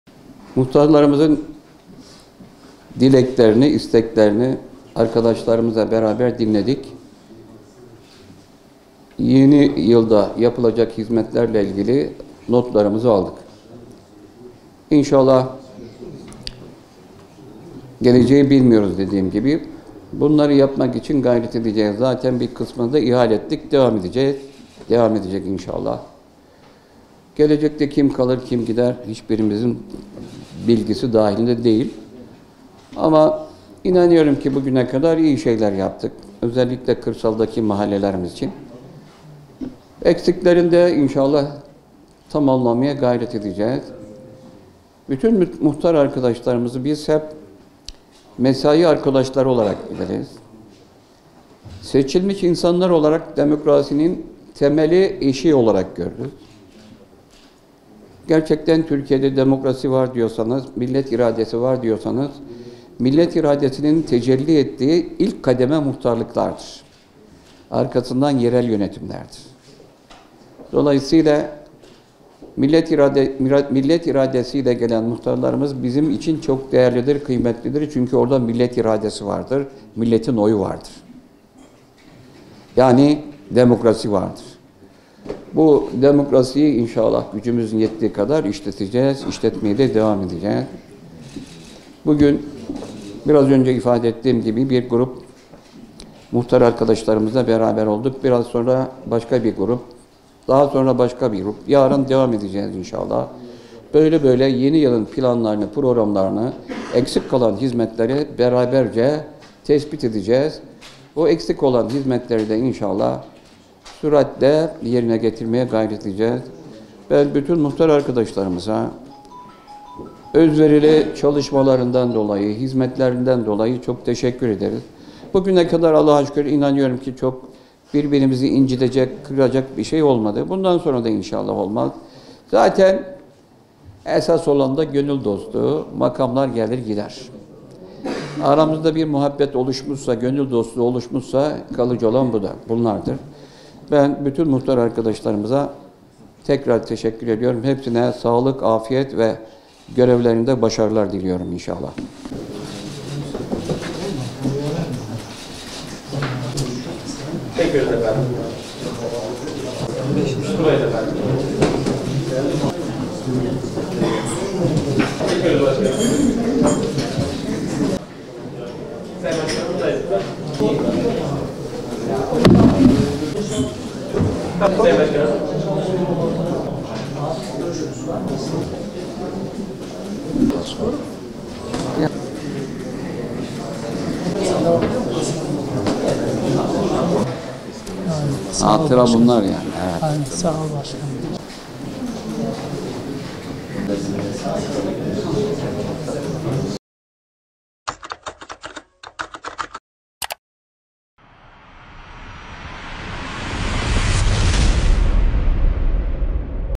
Başkan Mahçiçek; 100 yıllık hizmetleri 9 yılda Onikişubat’ımıza kazandırdık
Onikişubat Belediye Başkanı Hanefi Mahçiçek, 2023 Yılı İstişare ve Değerlendirme Toplantısı’nda mahalle muhtarlarıyla bir araya gelerek, yıl boyunca yapılan çalışmaları değerlendirdi. Sorun ve talepleri dinleyen Başkan Mahçiçek, 2024 yılı yatırım programının taleplere göre şekilleneceğini belirterek, “Hangi mahallemizin ne eksiği varsa telafi edeceğiz. Şunu da bilelim ki, 100 yıllık hizmetleri 9 yılda Onikişubat’ımıza kazandırdık” dedi.